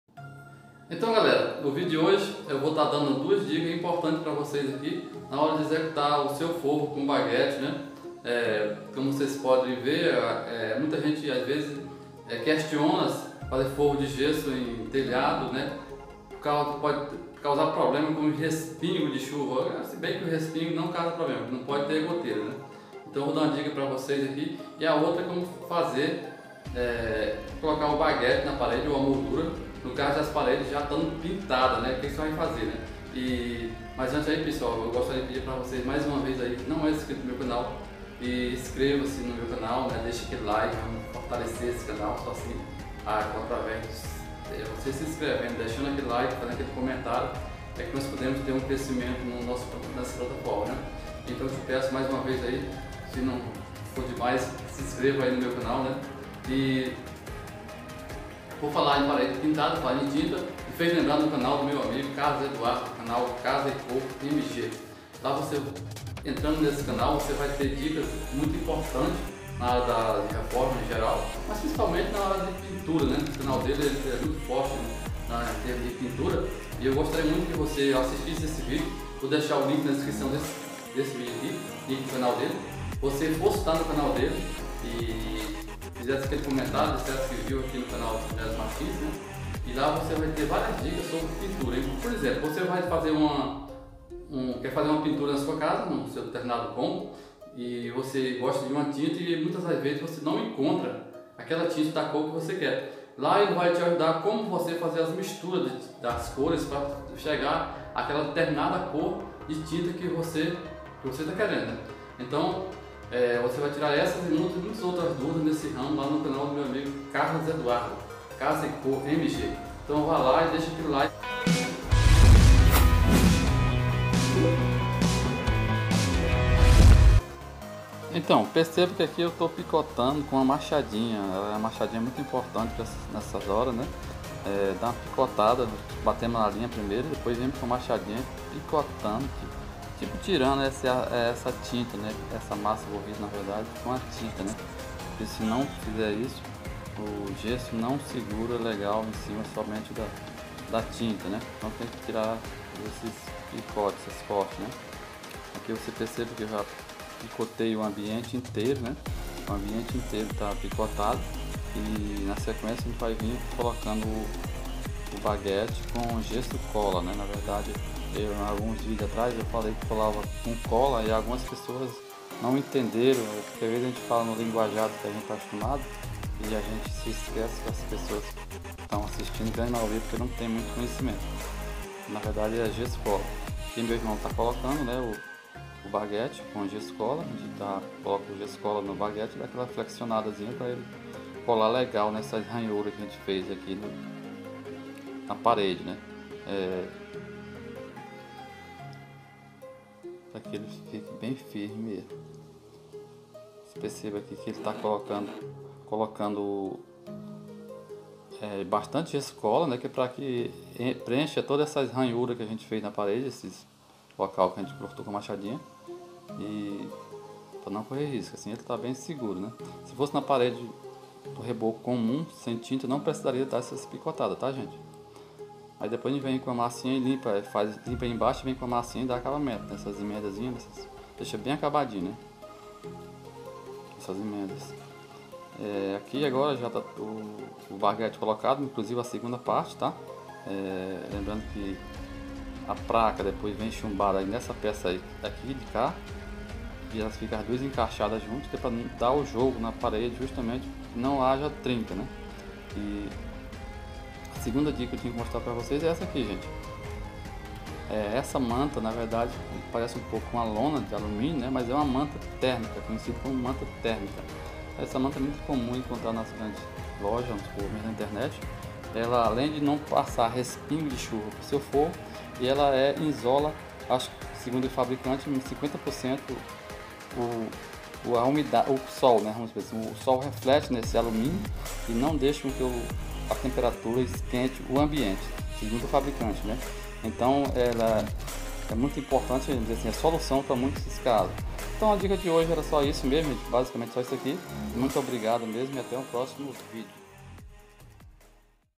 VC SABIA DESSA?!
Vc sabia que que tem que picota as paredes que já foram pintadas antes de colar o baguete de gesso ou moldura?,